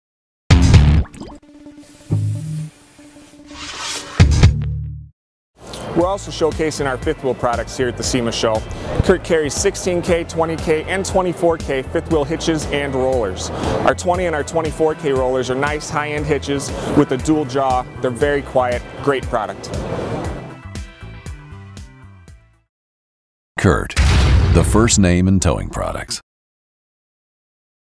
CURT - 5th Wheel Line
CURT 2013 specialized-towing products include 16K, 20K and 24K fifth wheel hitches and rollers. These high end, dual-jaw hitches are whisper-quiet and are designed to last.